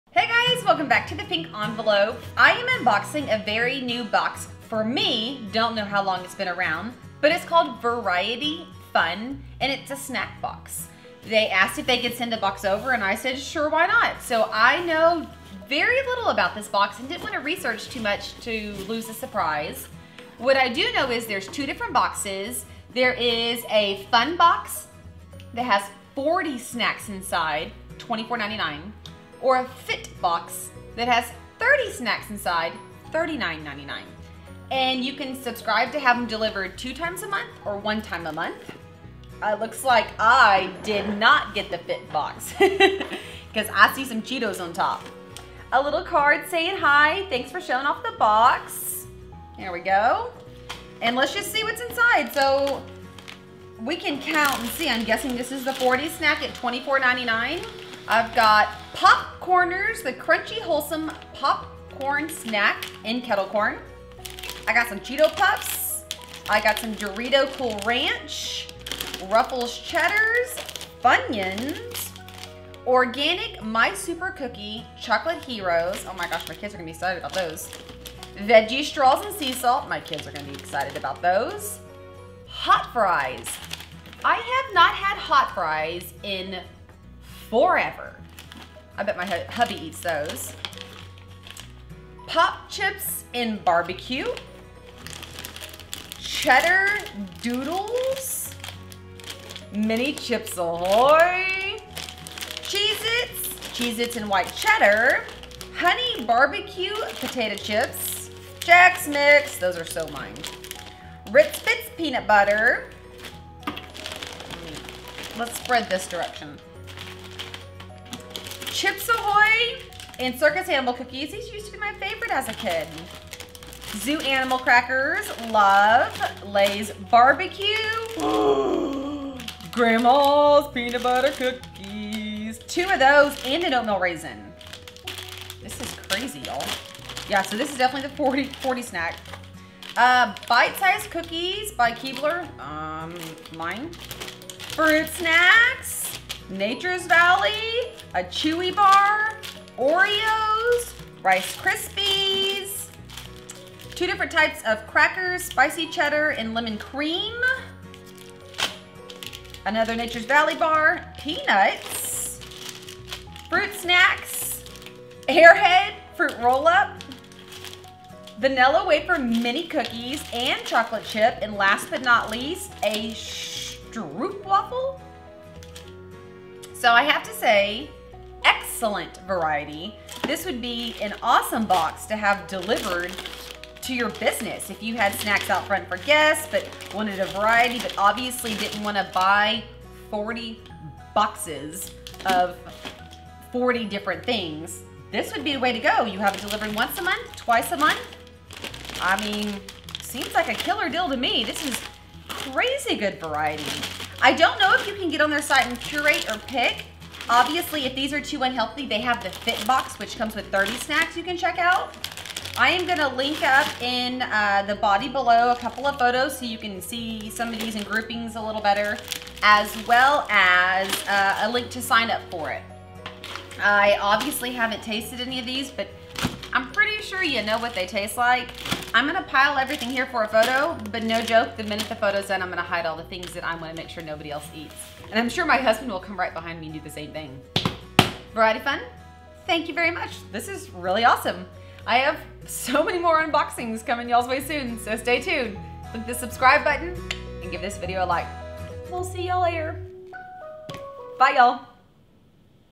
VarietyFun Review - Snack Subscription Box Unboxing
Smart, Delicious Snacks, Care Packages And Gifts Delivered Free.  They are Snacks Delivered to your Door.

Price: Fun Box with 40 snacks - $24.99 OR Fit Box with 30 snacks - $39.99
Frequency: 1 or 2 times a Month